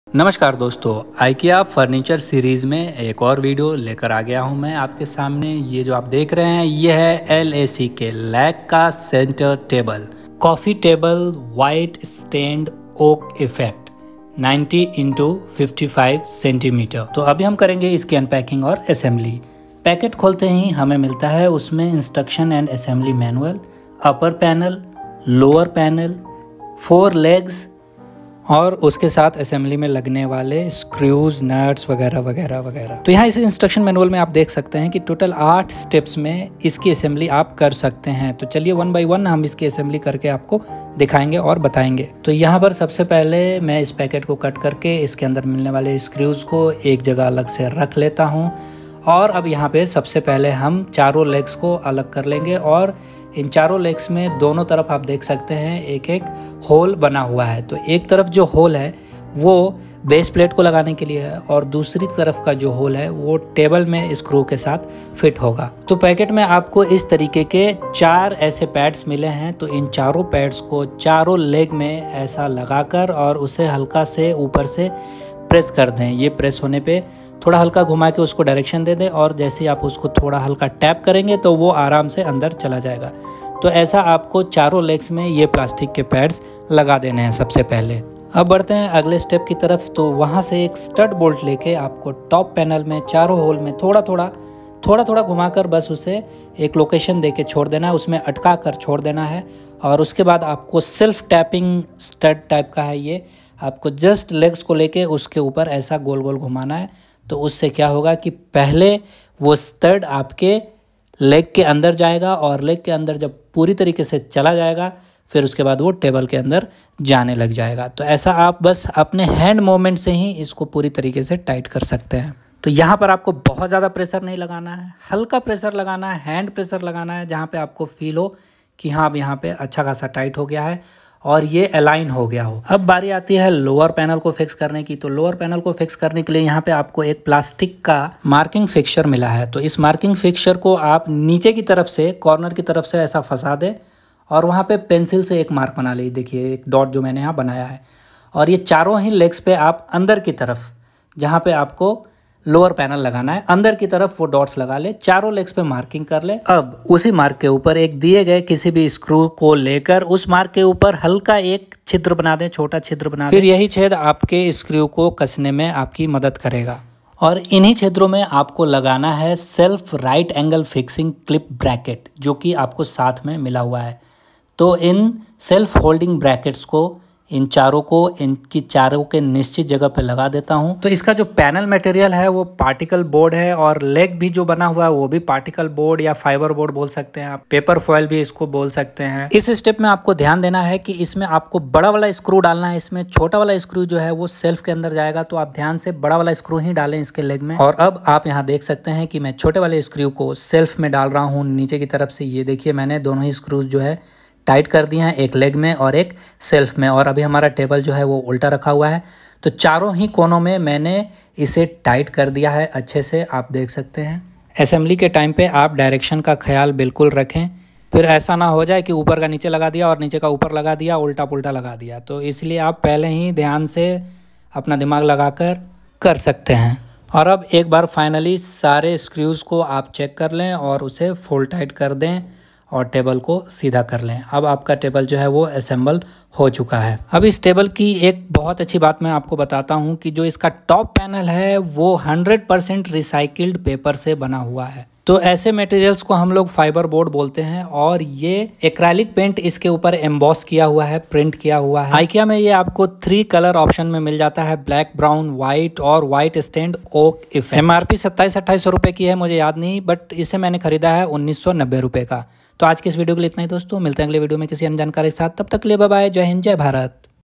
IKEA LACK Coffee table Assembly and Instruction
LACK Coffee table, white stained oak effect 90x55 cm (35 3/8x21 5/8 ")
Offer Price ₹1990/- incl. of all taxes
Separate shelf for magazines, etc. helps you keep your things organised and the table top clear.
Leg:
Particleboard, Fibreboard, Paper foil
Panel:
Particleboard, Printed and embossed acrylic paint, Melamine foil, Clear acrylic lacquer, Plastic edging
Top:
Particleboard, Honeycomb structure paper filling (100% recycled), Fibreboard, Printed and embossed acrylic paint, Clear acrylic lacquer, Plastic edging
Wipe clean with a damp cloth.
Wipe dry with a clean cloth.
Check regularly that all assembly fastenings are properly tightened and retighten when necessary.